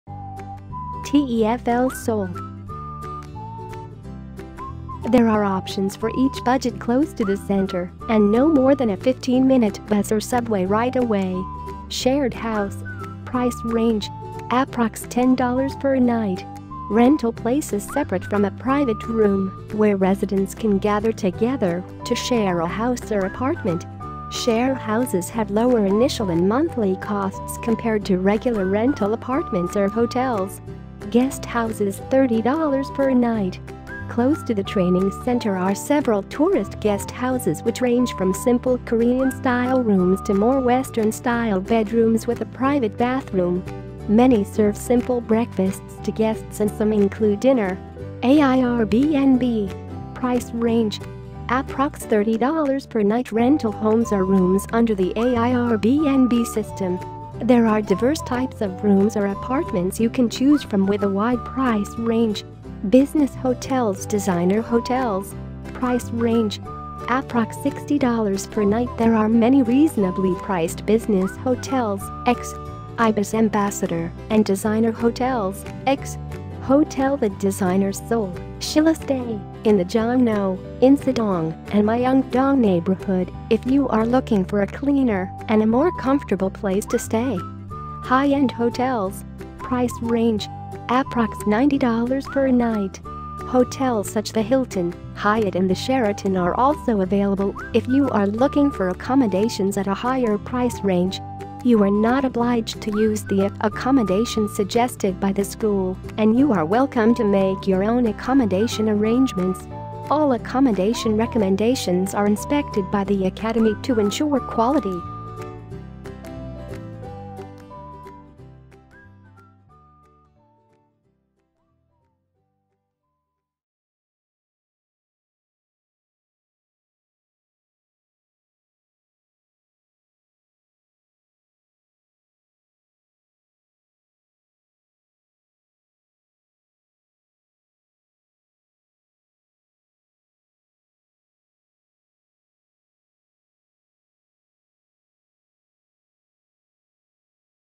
TEFL / TESOL School Accommodation in Seoul, South Korea | Teach & Live abroad!
TESOL Accommodation Seoul
There are options for each budget close to the centre and no more than a fifteen minute bus or subway ride away.

1. Shared House
Price range: approx $10 per night
Rental places separate from a private room where residents can gather together to share a house or apartment. Share houses have lower initial and monthly costs compared to regular rental apartments or hotels.

2. Guesthouses $30 per night
Close to the training centre are several tourist guesthouses which range from simple Korean style rooms to more western style bedrooms with a private bathroom. Many serve simple breakfasts to guests and some include dinner.

3. AIRBNB
Price range: approx $30 per night Rental homes or rooms under the AIRBNB system. There are diverse types of rooms or apartments you can choose from with a wide price range.

4. Business Hotels/Designer Hotels
Price range: approx $60 per night There are many reasonably priced business hotels (ex. IBIS Ambassador) and designer hotels(ex. Hotel The Designers Seoul, Shilla Stay)in the Jongno, Insadong, and Myungdong neighborhood if you are looking for a cleaner and a more comfortable place to stay.

5. High End Hotels
Price range: approx $90 per night

Hotels such the Hilton, Hyatt and the Sheraton are also available if you are looking for accommodations at a higher price range.

You are not obliged to use the accommodation suggested by the school and you are welcome to make your own accommodation arrangements. All accommodation recommendations are inspected by the academy to ensure quality.

Find out more about taking a TEFL course in Seoul